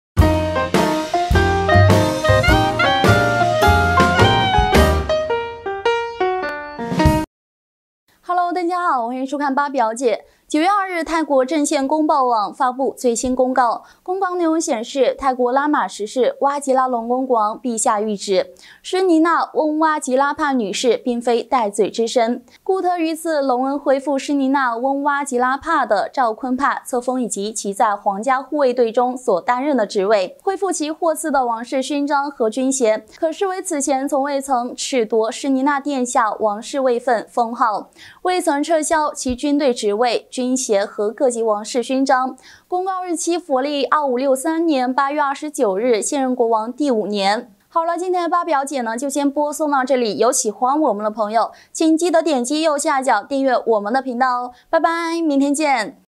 泰国贵妃正式复位！国王下圣旨恢复所有称号爵位，视同从未罢黜|扒表姐|@娱乐第一眼@八卦娱子酱@星闻速递#诗丽吉#泰王#苏提达
泰国贵妃正式复位！国王下圣旨恢复所有称号爵位，视同从未罢黜|扒表姐|@娱乐第一眼@八卦娱子酱@星闻速递